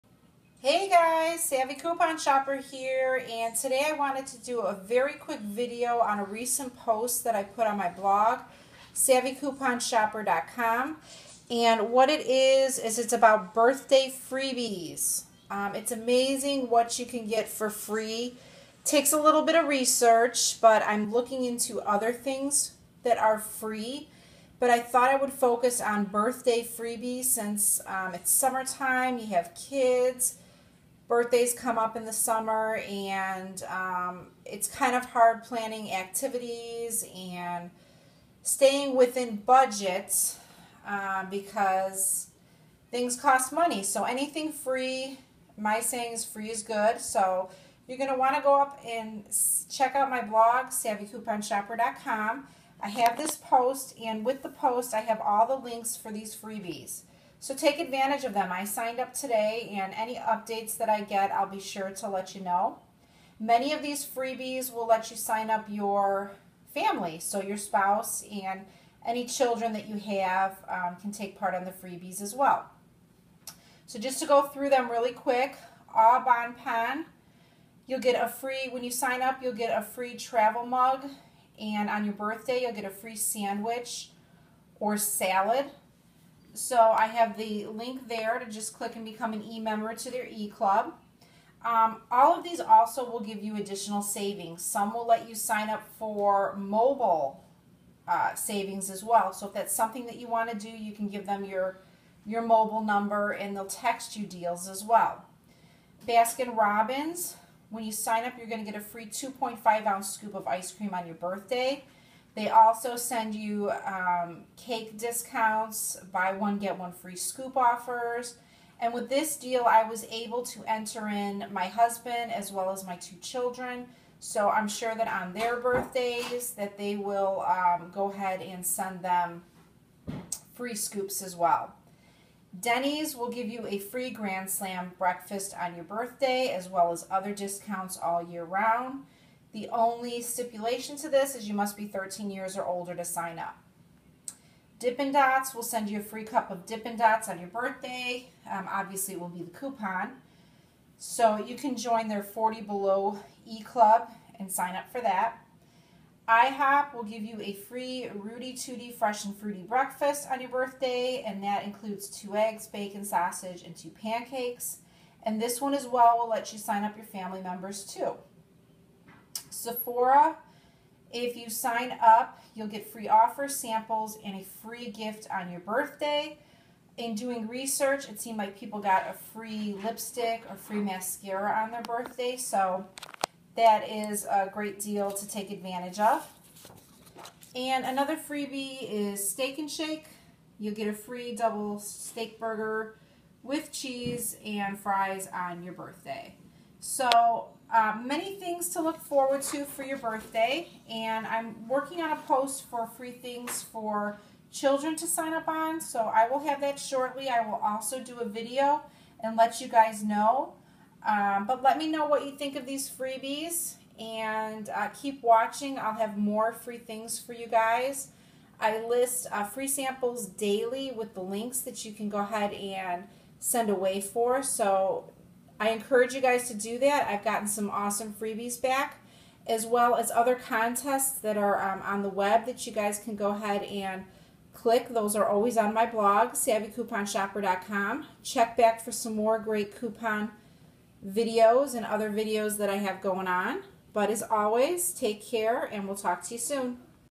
Awesome Freebies for your birthday!!!!! Sign-up today!!!!
There are so many FREE opportunities available for you on your birthday!!! Be sure to check out and sign-up for these awesome freebies today!